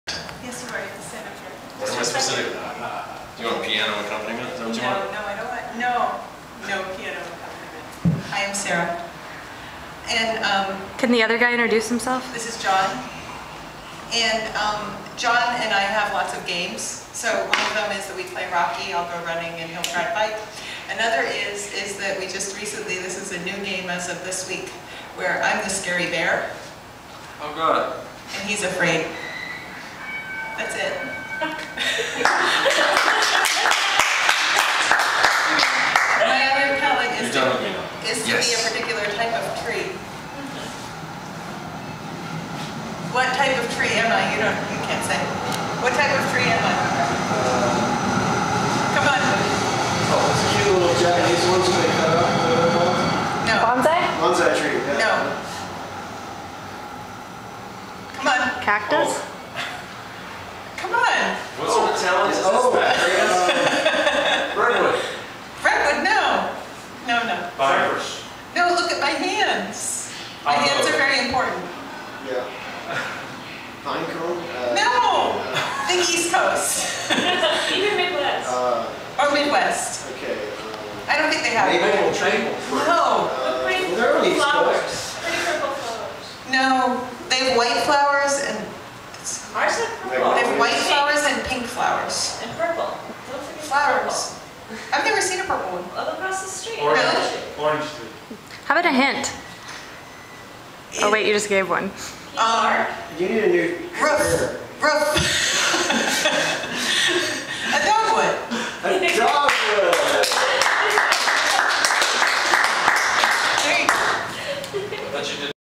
Sarah Wagner presents THE SCARY BEAR @ SBTC
live performance during one of the OPEN MIC / TALENT SHOWS here at the SOUTH BAY TALENT CENTER in San Jose (a San Jose Public Arts Commission project by Jon Brumit)